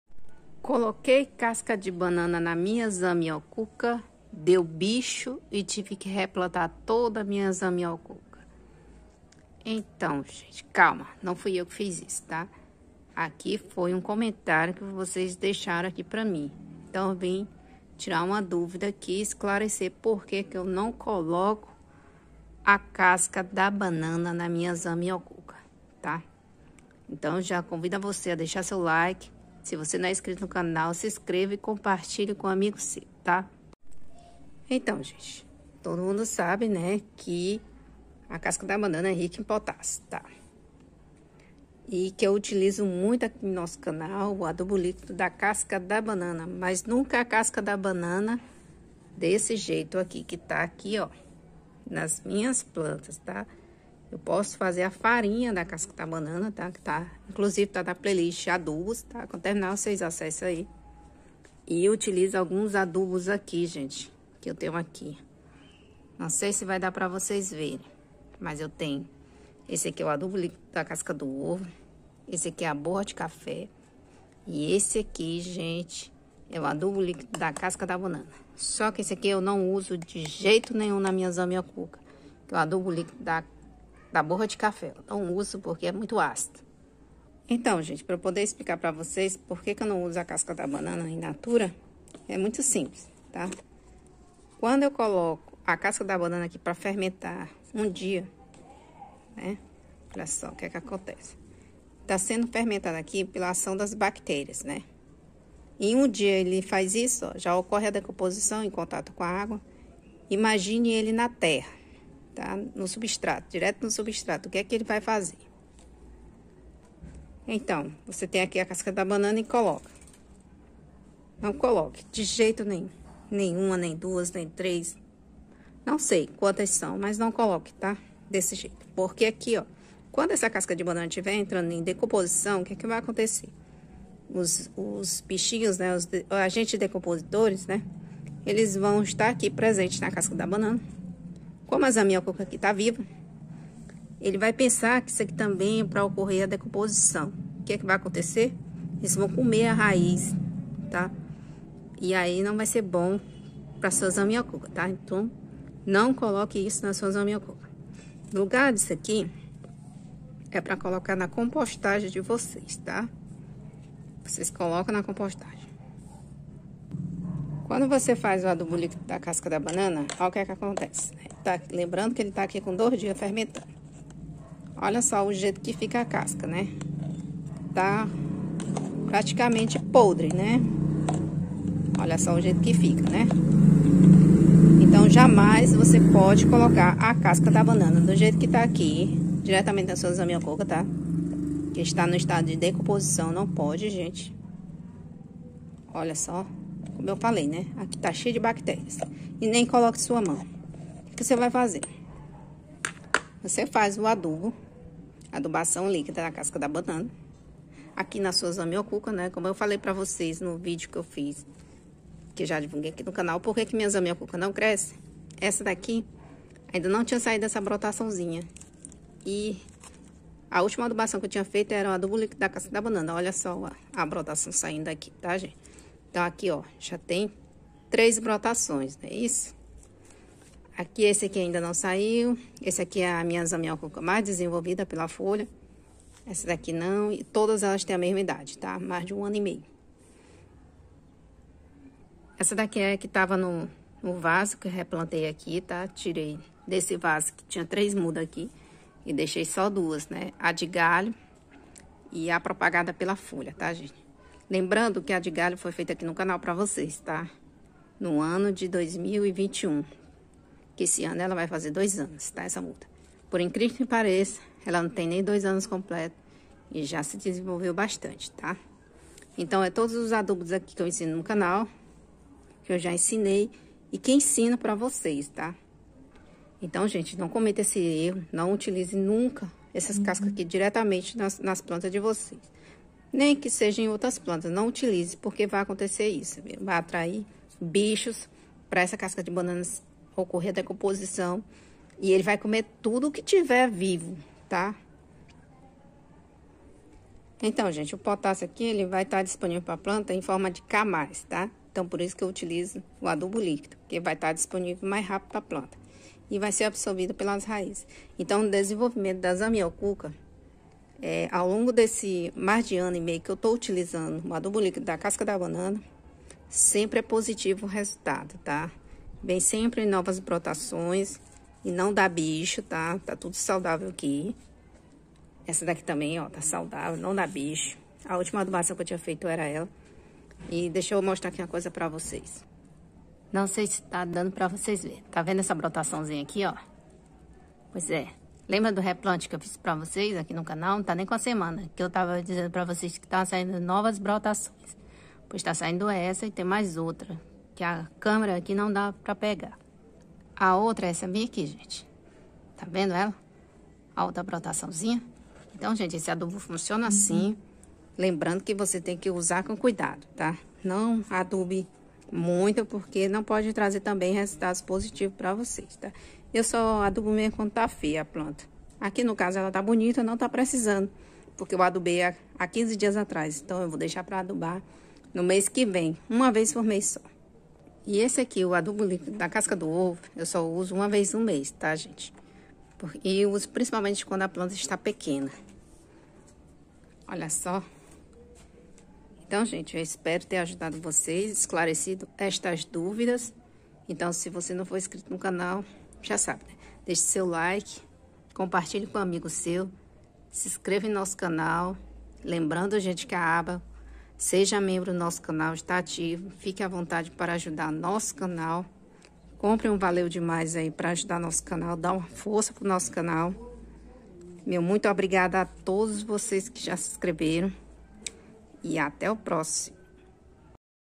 Casca de banana na zamioculca dá bicho??
00:00 início
1:32 porque não uso casca da banana In natura
3:37 mostrando as mudas, após adubação
4:43 não utilizar as cascas
5:00 potássio disponível para planta